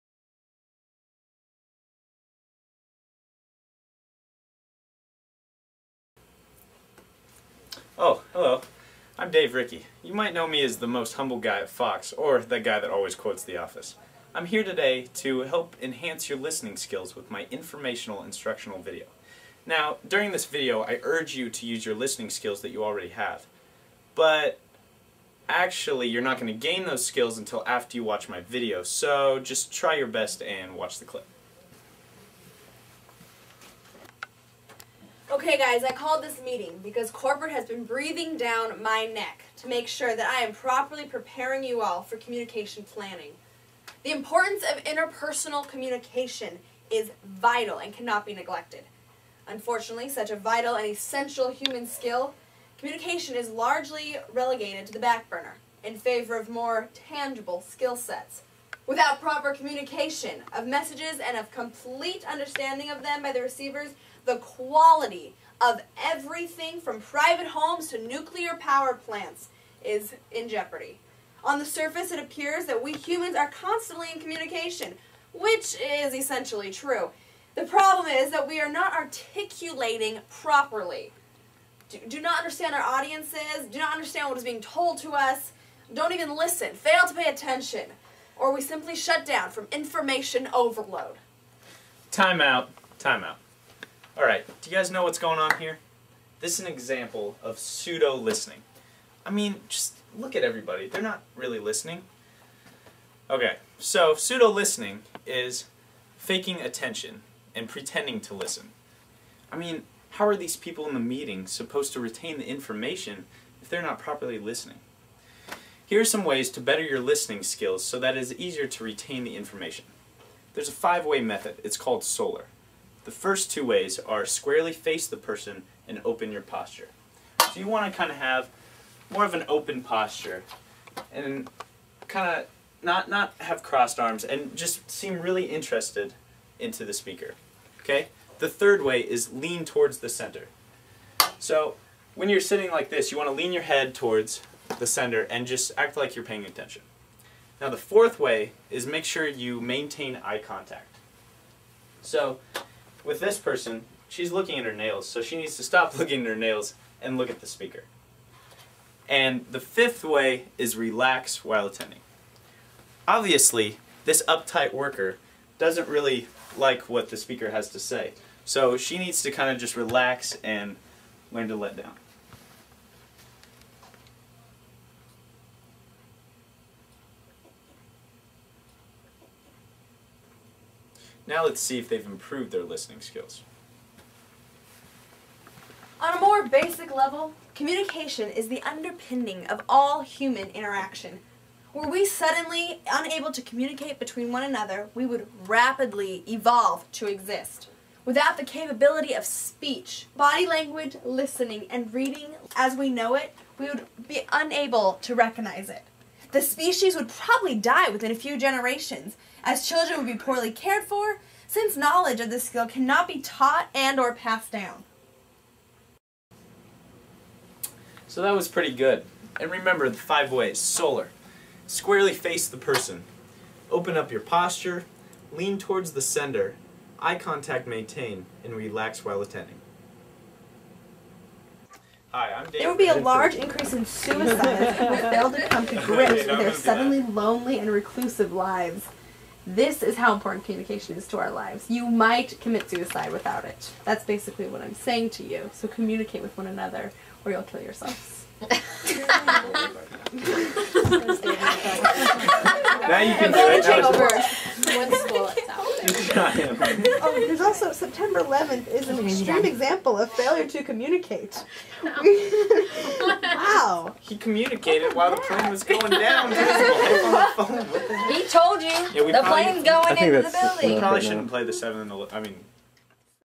Communication Shorts - Part 1 - Pseudo Listening
Dave Rickey teaches us about Pseudo Listening.